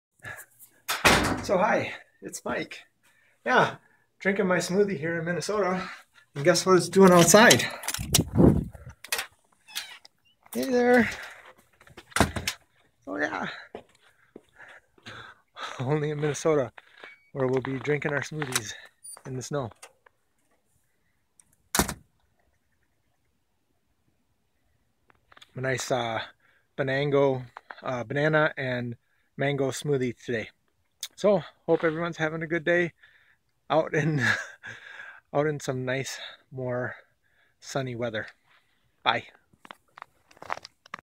Eating some raw lunch in the snow
Being raw in Minnesota means lots of room temperature smoothies for lunch.